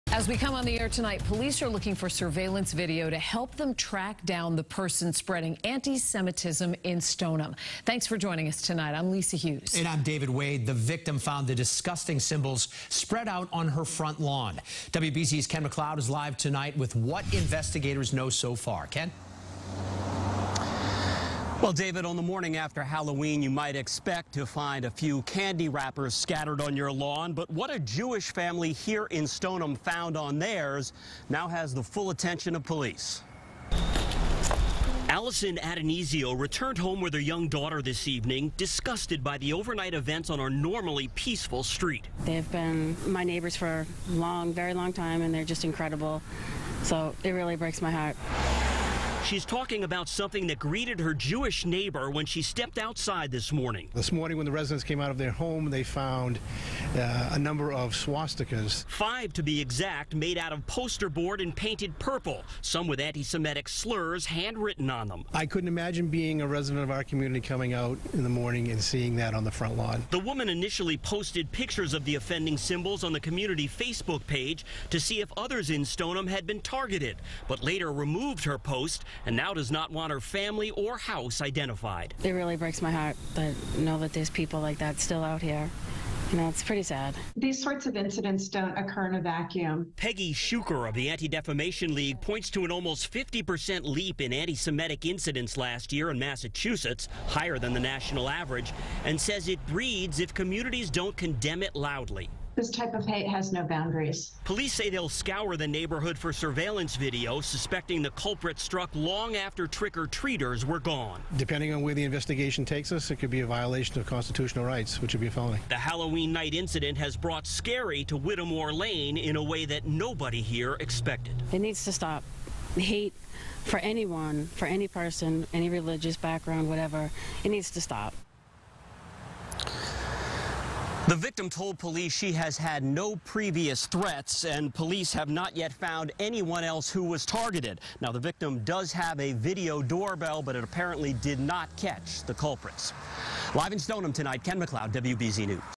Nazi symbols found outside Stoneham home
Police are investigating after swastikas were found on the lawn of a Stoneham home. WBZ-TV's Ken MacLeod reports.